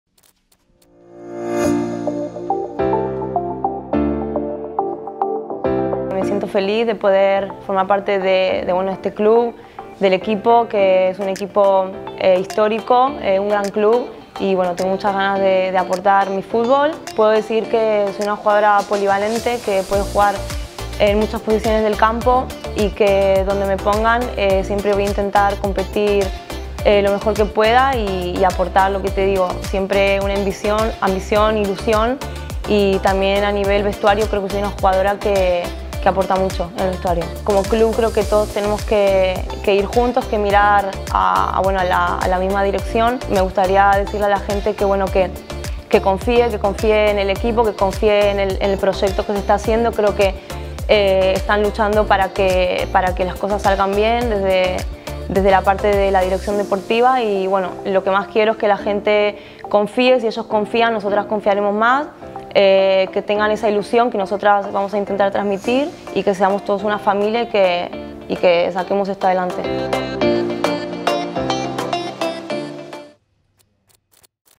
Marianela Szymanowski, nova jugadora de l'Espanyol!
Primeres declaracions de Marianela com a perica

Primeras declaraciones de Marianela como perica